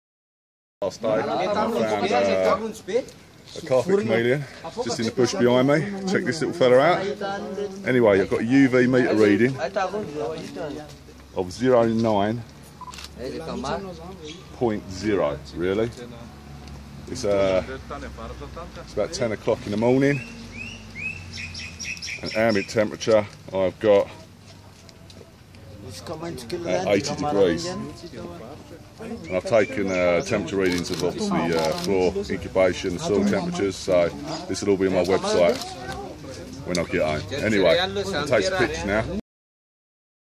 Millennium Reptiles discovers a wild Carpet Chameleon in Madagascar!
Millennium Reptiles discovers Madagascar in search of some amazing reptiles! Here Lee has found a wild Carpet Chameleon (Furcifer Lateralis)

Millennium Reptiles
Unit D
The Maltings
Station Road
Sawbridgeworth
Hertfordshire
CM21 9JX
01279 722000